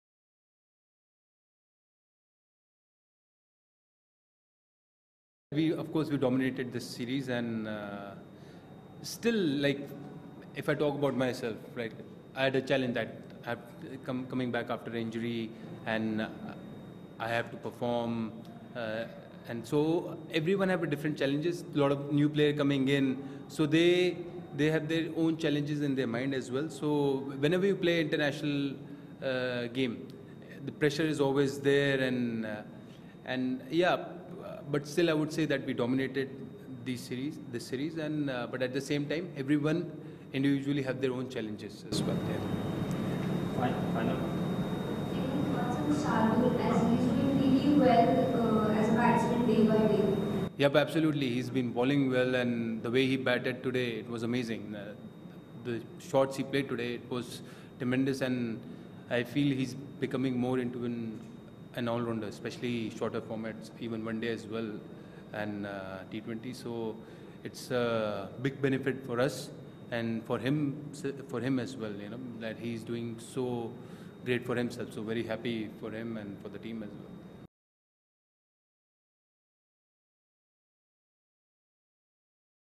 We dominated the series: Shikhar Dhawan after victory over Sri Lanka
Pune (Maharashtra), Jan 11 (ANI): After winning the three-match T20I series against Sri Lanka India batsman Shikhar Dhawan said they dominated the series while experimenting with different players and batting order. "We dominated the series and if I talk about myself, I had a challenge that I am coming back after an injury and I have to perform. Everyone has different challenges. A lot of players coming in, they have their own challenges," Dhawan said in the post-match press conference.